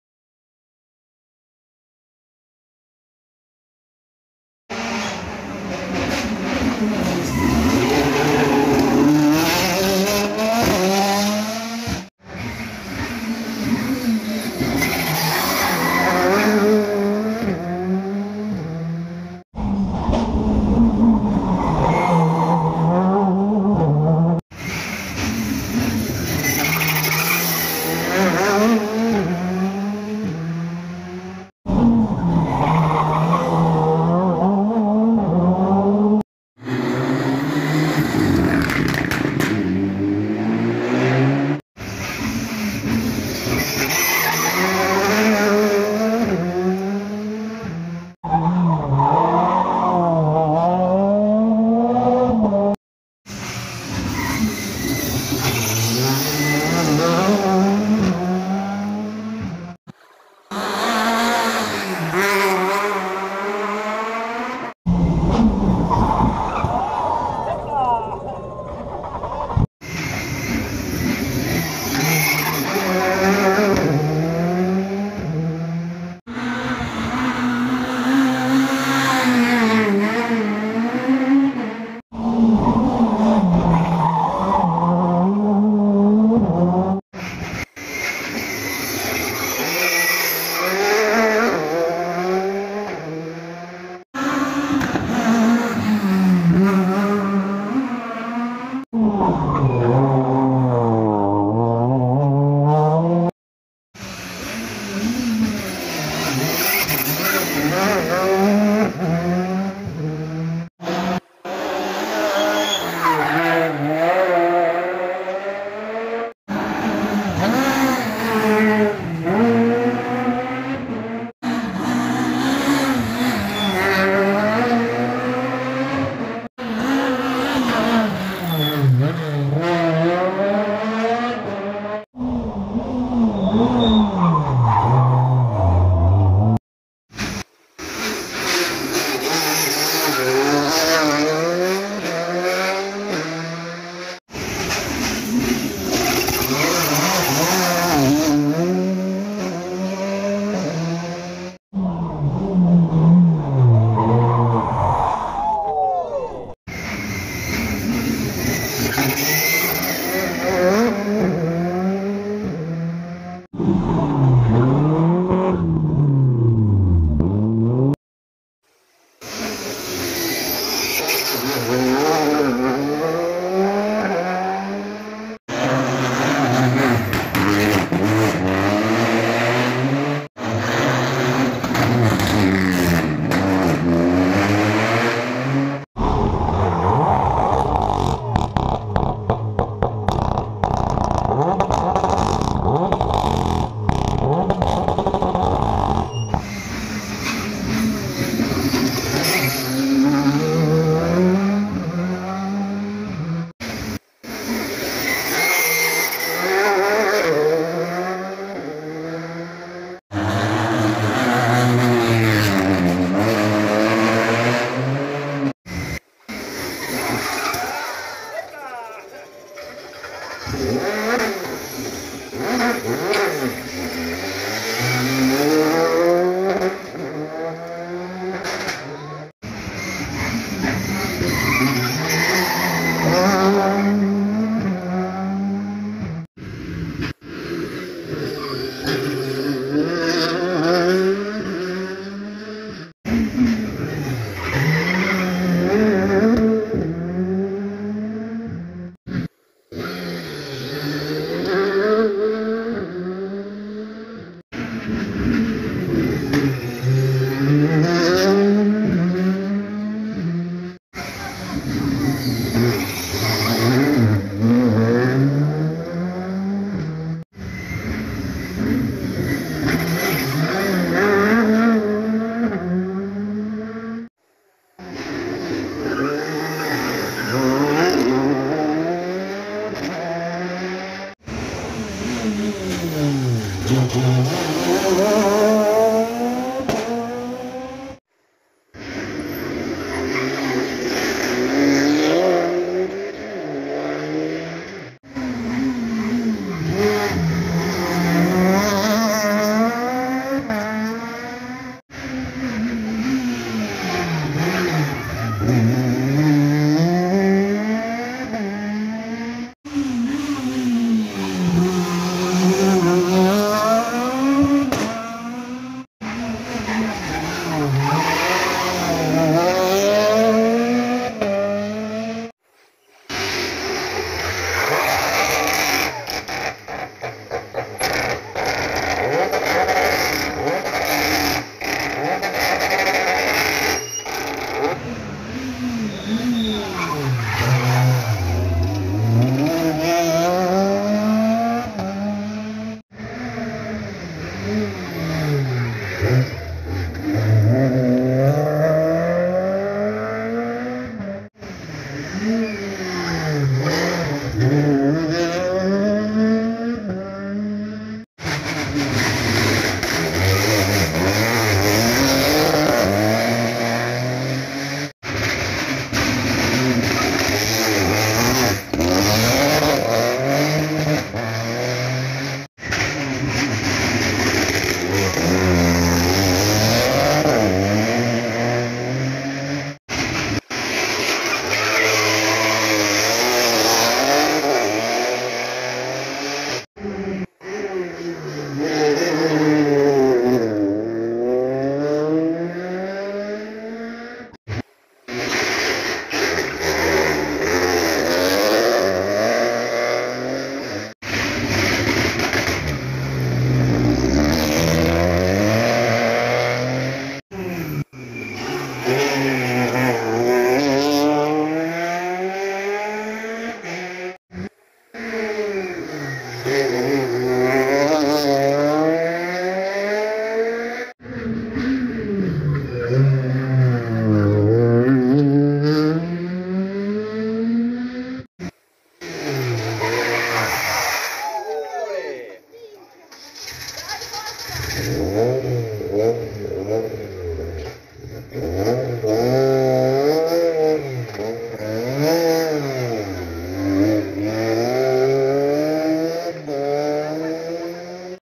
58° Rally Valli Ossolane 2022 Mistakes e show!!
mix passaggi della ps Vigezzo